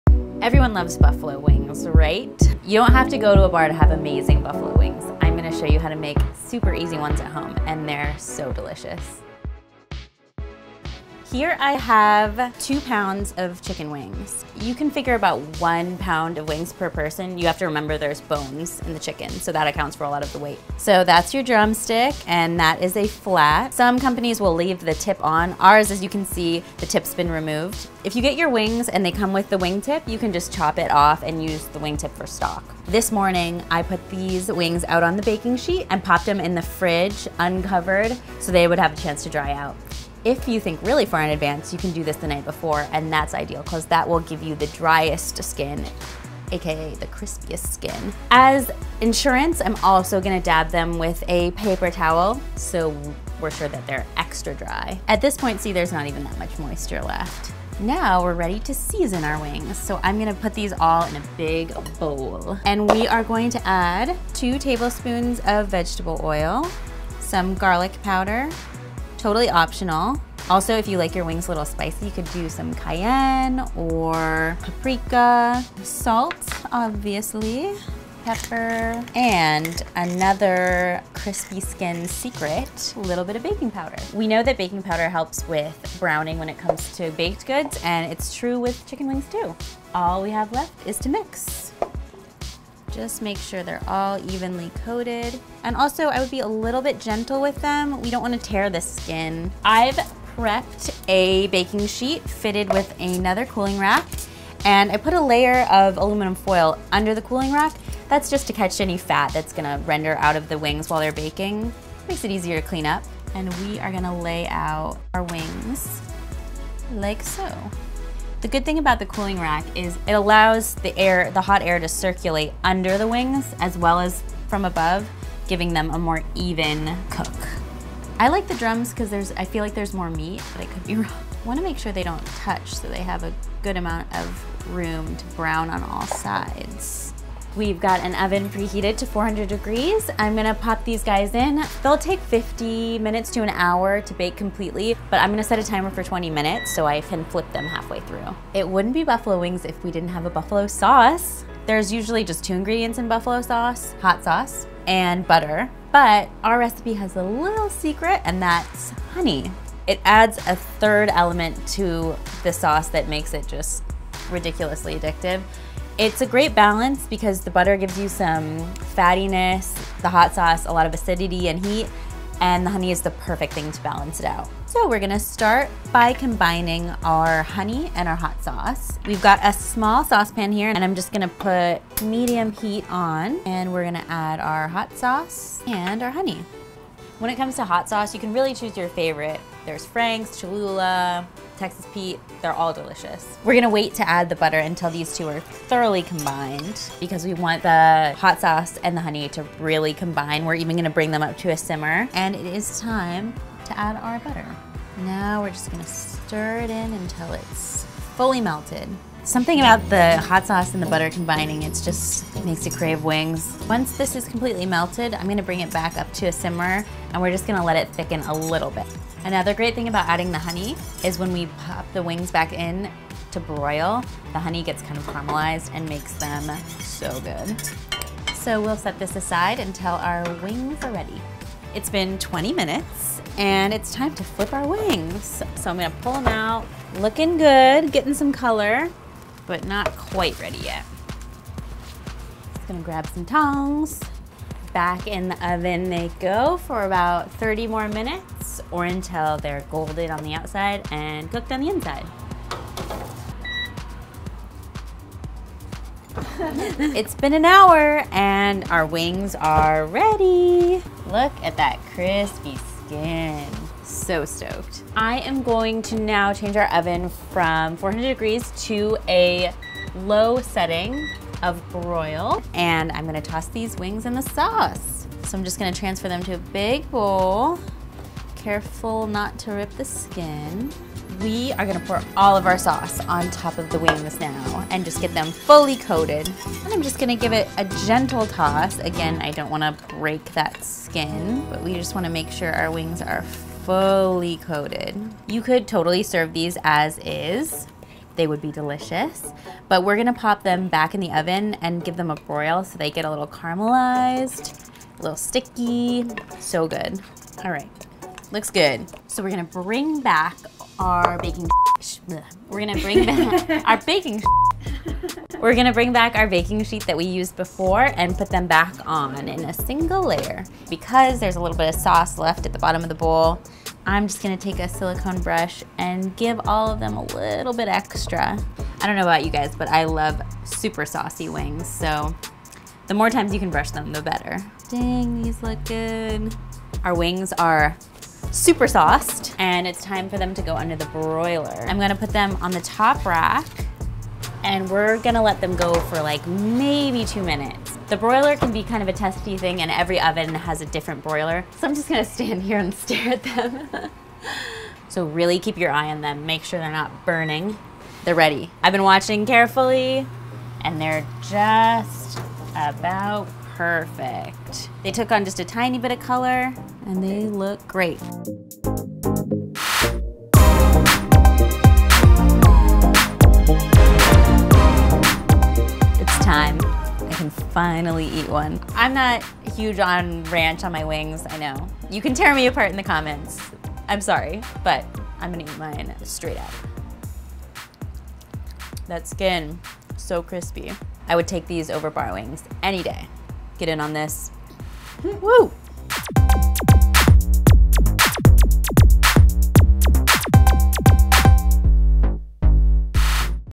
How To Make The Crispiest Baked Buffalo Chicken Wings | Delish
Leave fried wings to the pub. It's messy and involved, and not what you want to be doing when the game is on. Baking the chicken is wayyy easier.